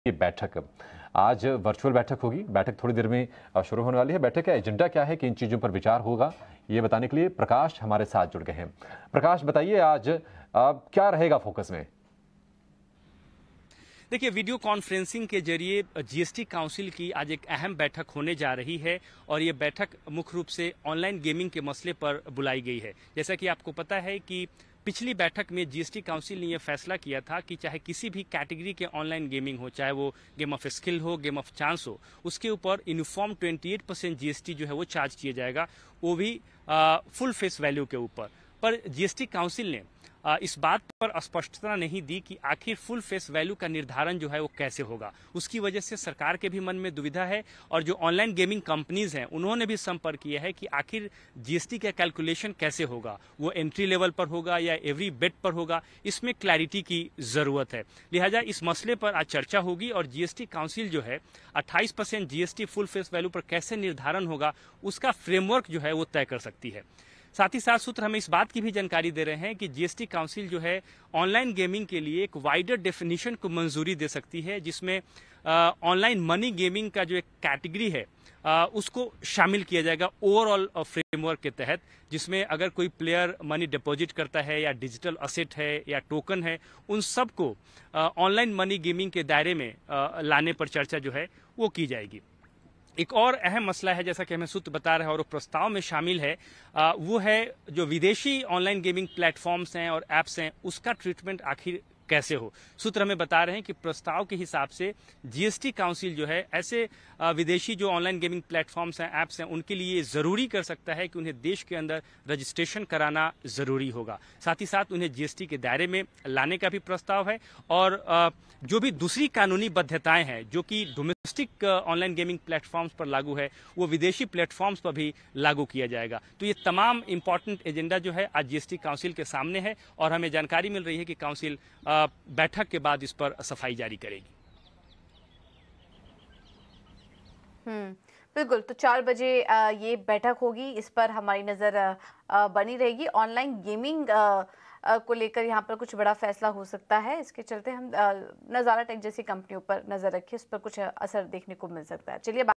GST Council की Meeting में Online Gaming Tax पर चर्चा आज, 28% GST लगाने की क्या होगी रूपरेखा? | ETNS
GST Council Meeting on Online Gaming : जीएसटी काउंसिल की आज वर्चुअल बैठक होगी, मीटिंग में अहम मुद्दा ऑनलाइन गेमिंग पर GST Tax को लेकर है। बता दें पिछली बैठक में कहा गया था कि किसी भी गेम पर 28% का जीएसटी लगेगा। लेकिन ये बात साफ नहीं थी कि जीएसटी लगाने की क्या रुपरेखा होगी। आज बैठक में Online Gaming के फेस वैल्यू फ्रेमवर्क पर चर्चा होगी।